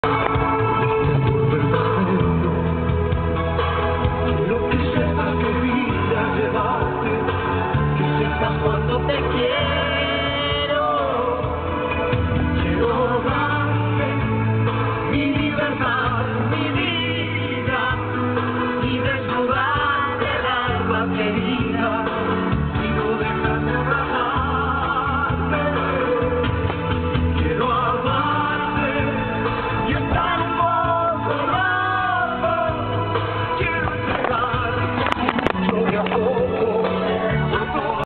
Navidad III a todo sergio
Final de la noche navideña en el tutu de marinha escuchando sergio denis